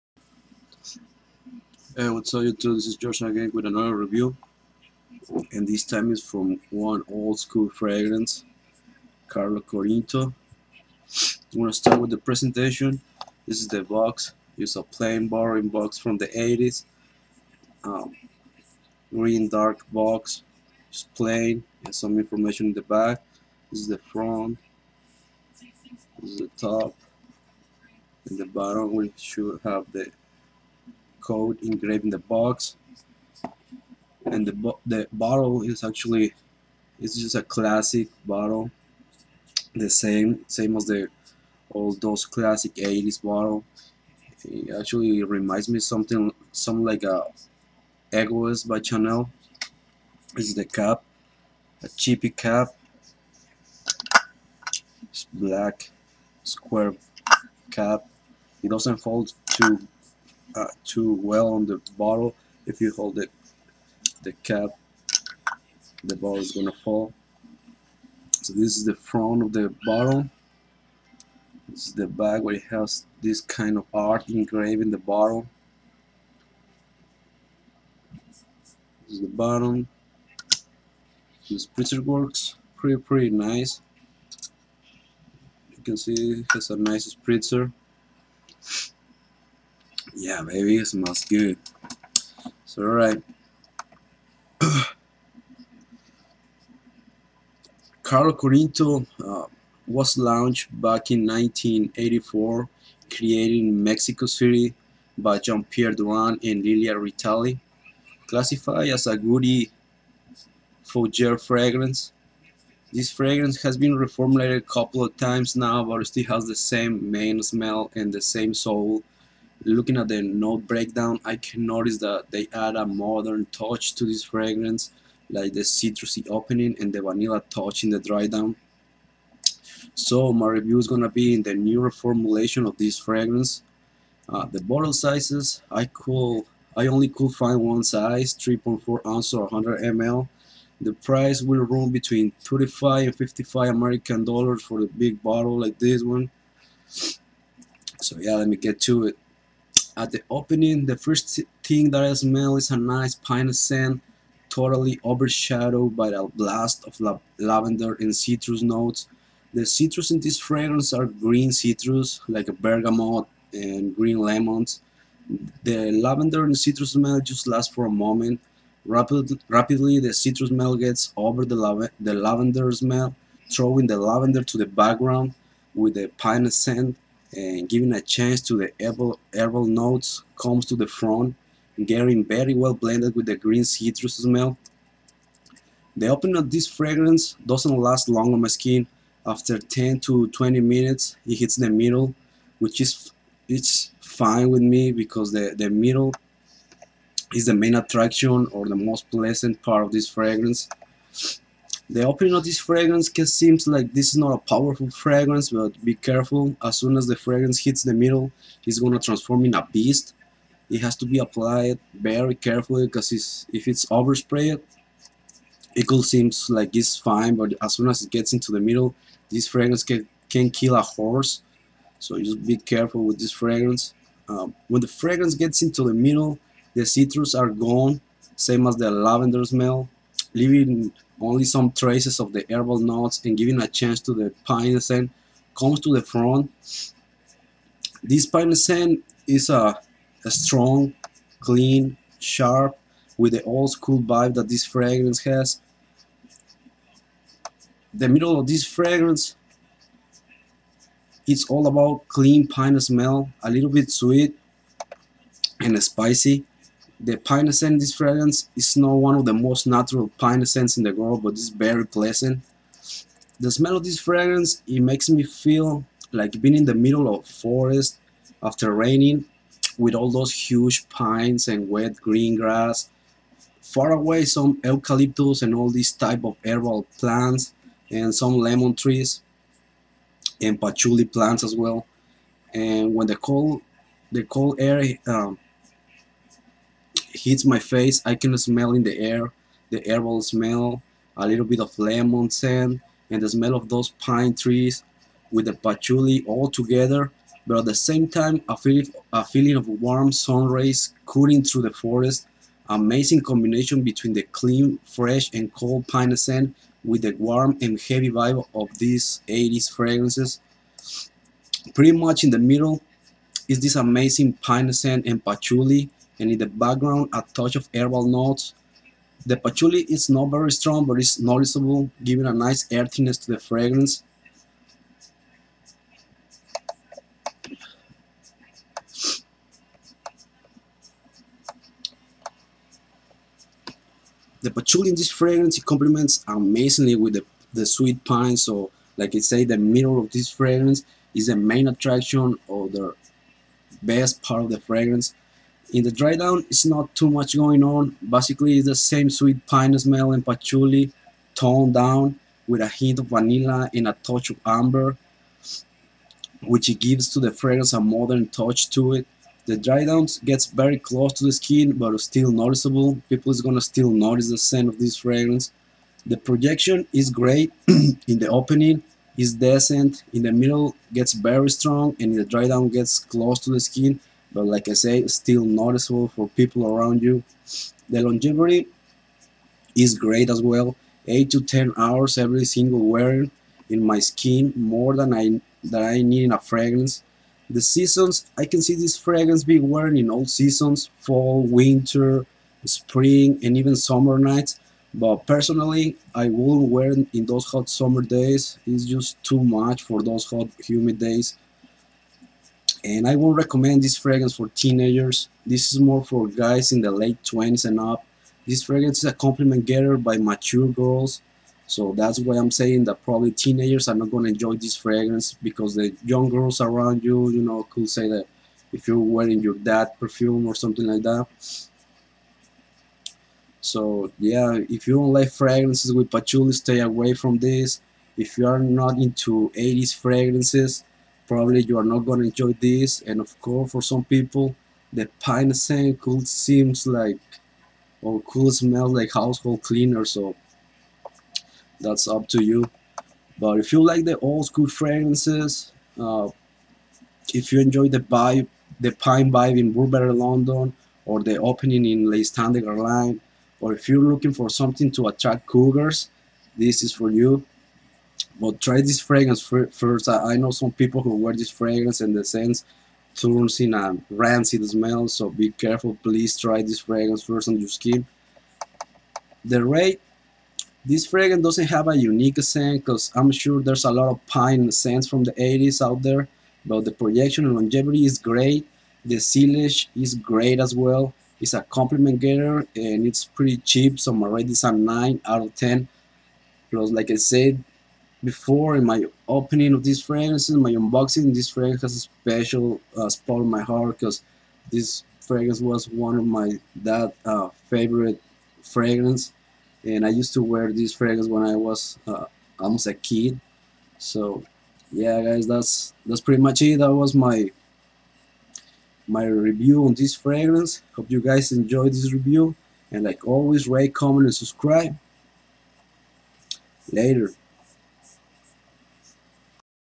Carlo Corinto fragrance review (part no.2)
my take on this amazing fragrance from the 80's, perfect for all those mature guys out there and perfect for all seasons